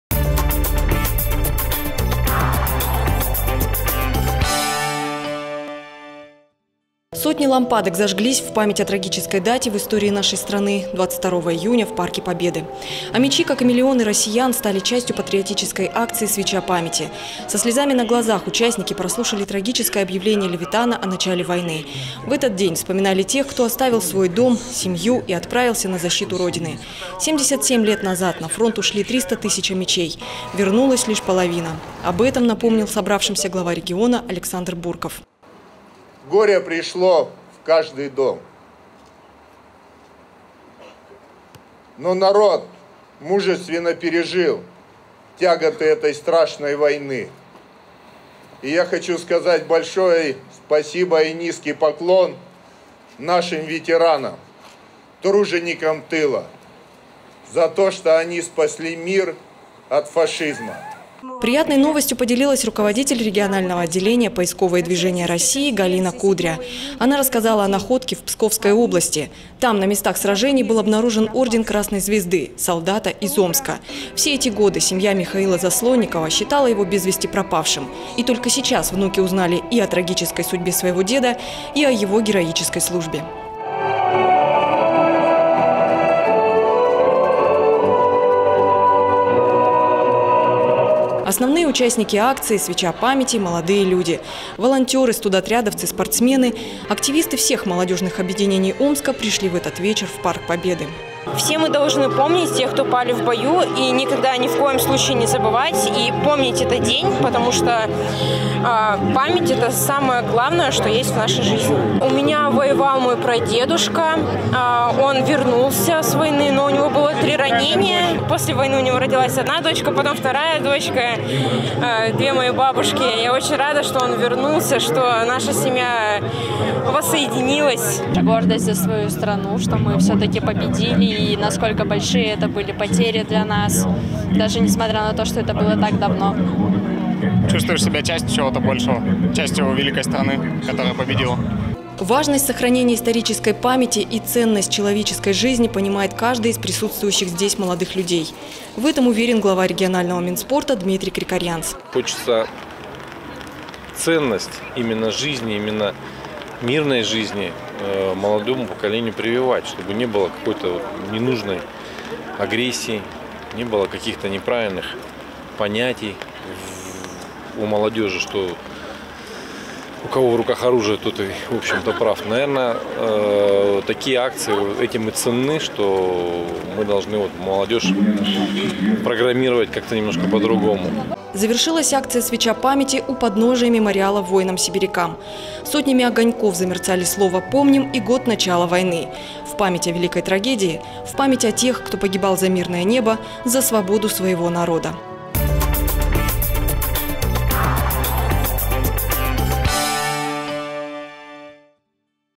Всероссийская акция «Свеча памяти»
22 июня в парке Победы состоялась Всероссийская акция «Свеча памяти»